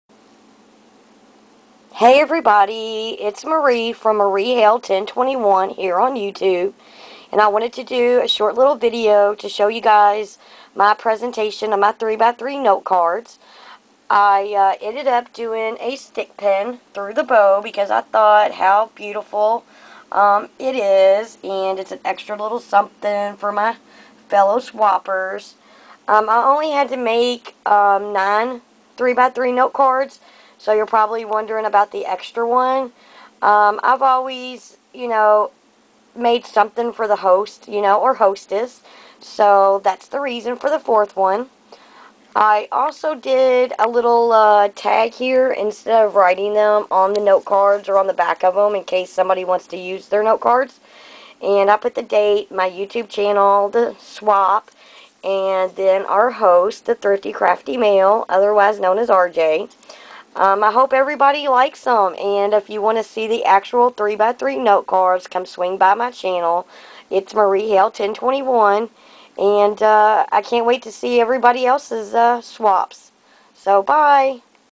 3x3 NOTE CARD SWAP 2013
this is a video of my final presentation of my 3x3 note card swap over at :
So go check him out and his wonderful videos